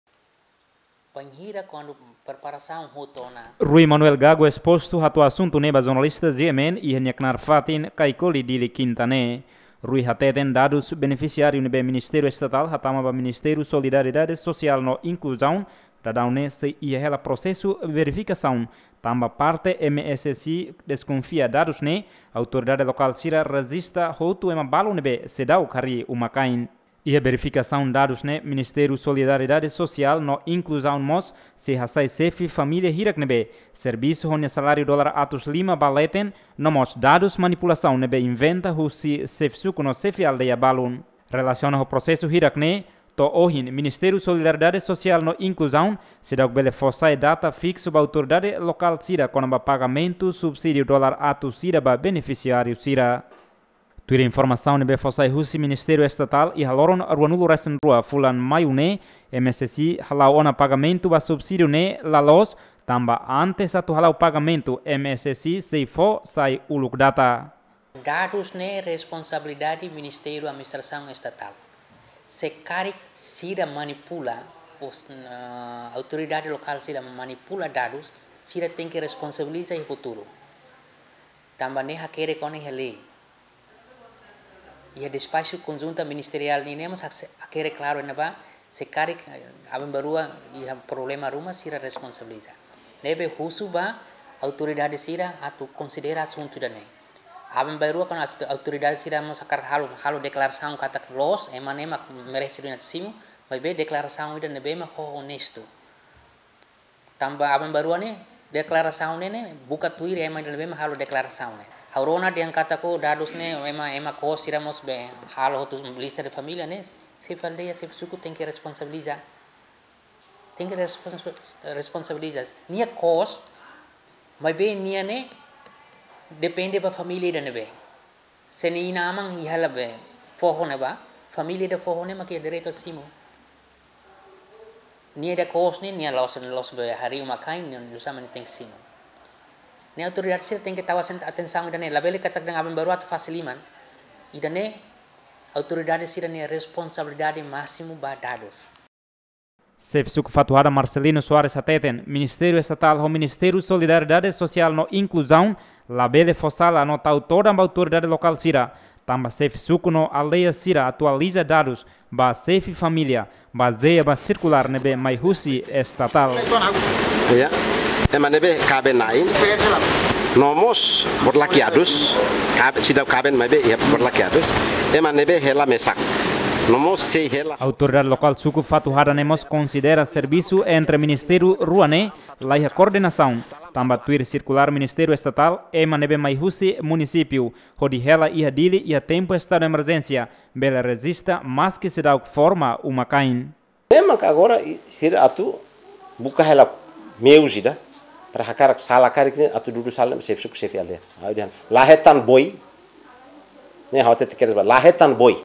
MSSI Seidauk bele fó sai Data Fixu ba Autoridade local sira konaba Pagamentu Subsídiu $100
MSSI Seidauk bele fó sai Data Fixu ba Autoridade local sira konaba Pagamentu Subsídiu $100 ba Benefisiariu sira